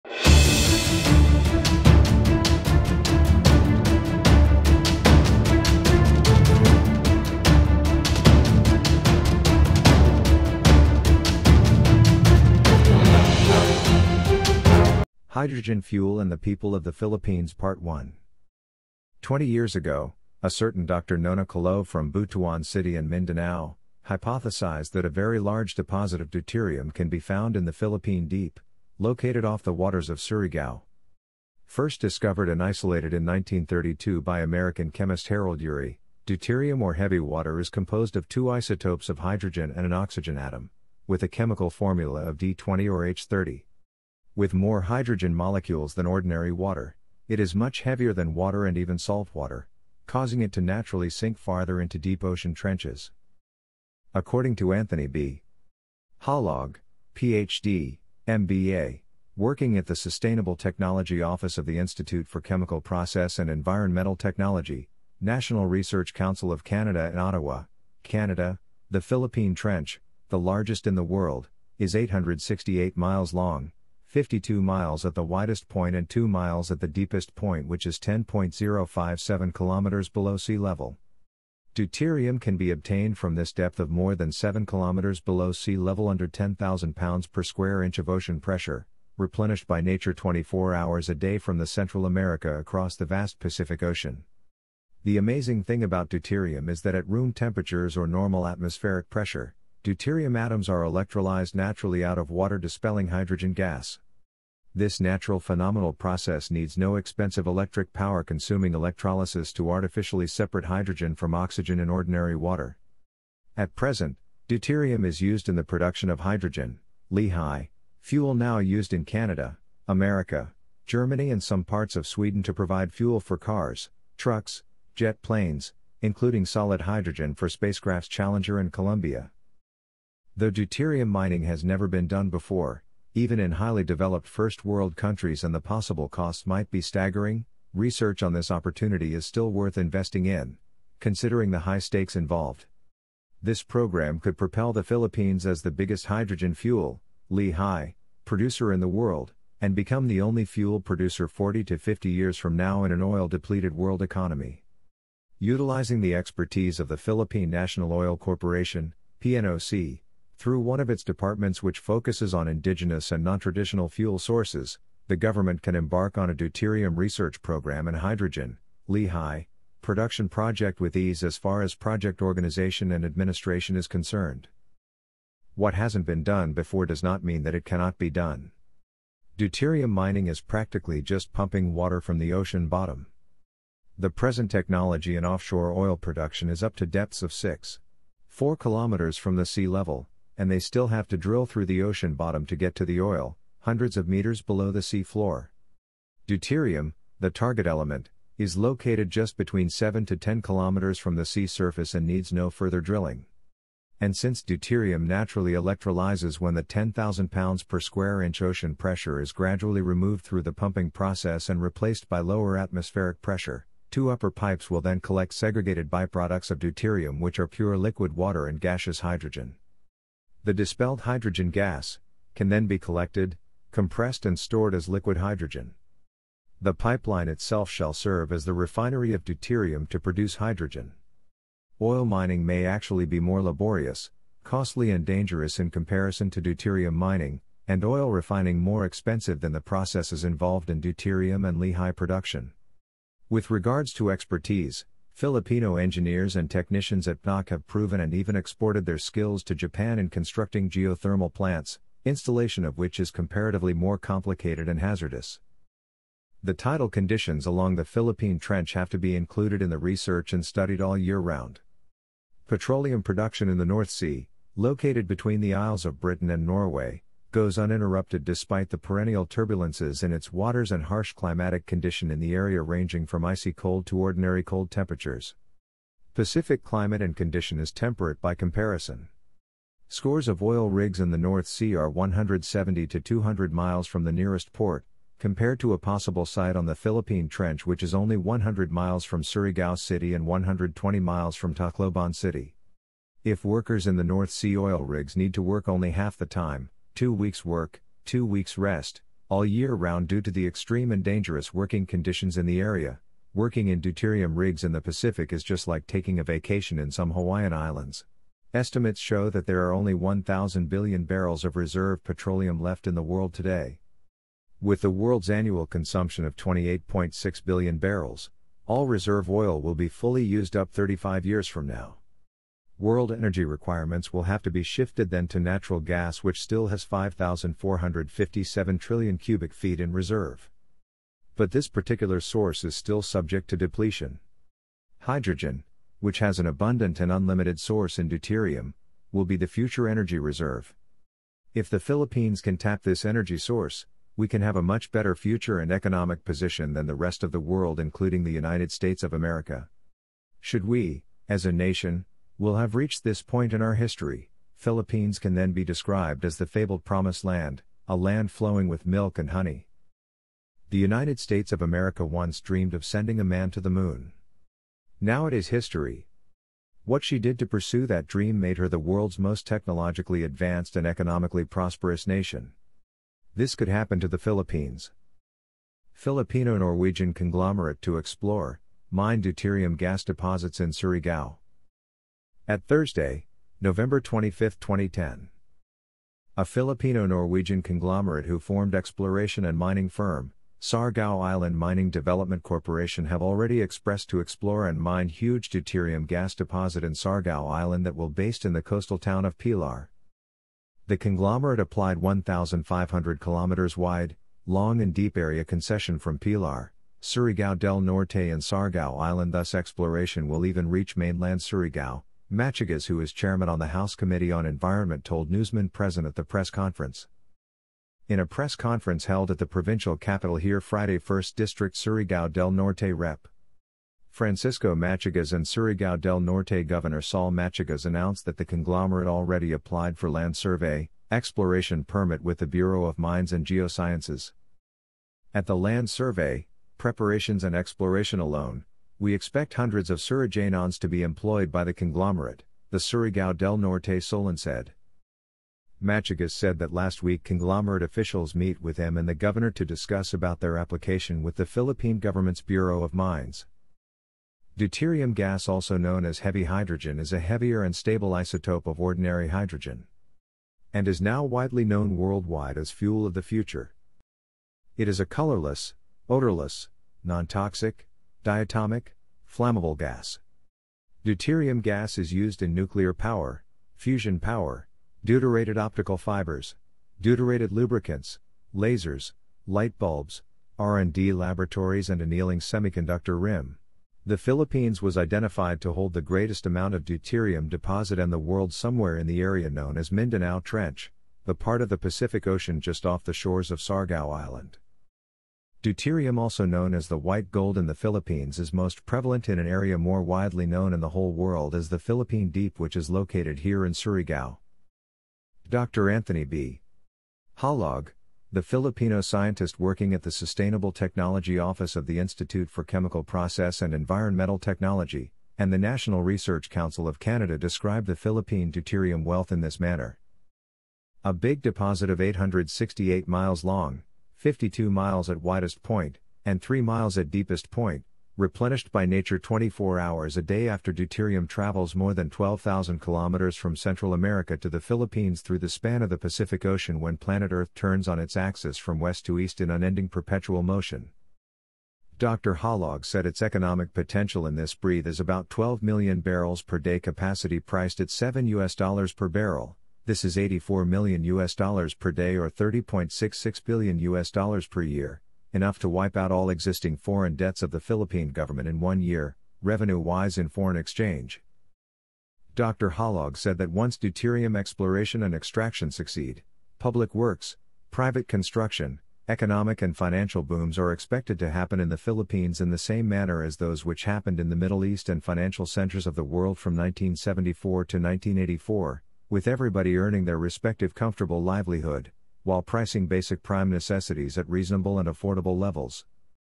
Hydrogen FUEL And The People Of The Philippines Part 2
The mystery chapter of Deuterium Gas Part 2

Twenty years ago, a certain Dr. Nona Calo from Butuan City in Mindanao, hypothesized that a very large deposit of deuterium can be found in the Philippine Deep, located off the waters of Surigao.  First discovered and isolated in 1932 by an American chemist Harold Urey, deuterium or heavy water is composed of two isotopes of hydrogen and an oxygen atom, with a chemical formula of D20 or H30.  With more hydrogen molecules than ordinary water, it is much heavier than water and even saltwater, causing it to naturally sink farther into deep ocean trenches.

          According to Anthony B. Halog (PhD, MBA), working at the Sustainable Technology Office of the Institute for Chemical Process and Environmental Technology, National Research Council of Canada in Ottawa, Canada, "the Philippine Trench, the largest in the world, is 868 miles long, 52 miles at the widest point and 2 miles at the deepest point which is 10.057 kilometers below sea level.  Deuterium can be obtained from this depth of more than 7 kilometers below sea level under 10,000 psi of ocean pressure, replenished by nature 24 hours a day from the Central America across the vast Pacific Ocean.  The amazing thing about deuterium is that at room temperatures or normal atmospheric pressure, deuterium atoms are electrolyzed naturally out of water dispelling hydrogen gas.  This natural phenomenal process needs no expensive electric power-consuming electrolysis to artificially separate hydrogen from oxygen in ordinary water.  At present, deuterium is used in the production of hydrogen (Li-Hy) fuel now used in Canada, America, Germany and some parts of Sweden to provide fuel for cars, trucks, jet planes, including solid hydrogen for spacecrafts Challenger and Columbia".

          Though deuterium mining has never been done before, even in highly developed first world countries and the possible costs might be staggering, research on this opportunity is still worth investing in, considering the high stakes involved.  This program could propel the Philippines as the biggest hydrogen fuel (Li-Hy) producer in the world, and become the only fuel producer 40 to 50 years from now in an oil-depleted world economy.  Utilizing the expertise of the Philippine National Oil Corporation (PNOC) through one of its departments which focuses on indigenous and non-traditional fuel sources, the government can embark on a deuterium research program and hydrogen (Li-Hy) production project with ease as far as project organization and administration is concerned.

          "What hasn't been done before does not mean that it cannot be done.  Deuterium mining is practically just pumping water from the ocean bottom.  The present technology in offshore oil production is up to depths of 6.4 kilometers from the sea level, and they still have to drill through the ocean bottom to get to the oil, hundreds of meters below the sea floor.  Deuterium, the target element, is located just between 7 to 10 kilometers from the sea surface and needs no further drilling.  And since deuterium naturally electrolyzes when the 10,000 psi ocean pressure is gradually removed through the pumping process and replaced by lower atmospheric pressure, two upper pipes will then collect  segregated by-products of deuterium which are pure liquid water and gaseous hydrogen.  The dispelled hydrogen gas, can then be collected, compressed and stored as liquid hydrogen.  The pipeline itself shall serve as the refinery of deuterium to produce hydrogen.  Oil mining may actually be more laborious, costly and dangerous in comparison to deuterium mining, and oil refining more expensive than the processes involved in deuterium and LiHy production.  With regards to expertise, Filipino engineers and technicians at PNOC have proven and even exported their skills to Japan in constructing geothermal plants, installation of which is comparatively more complicated and hazardous.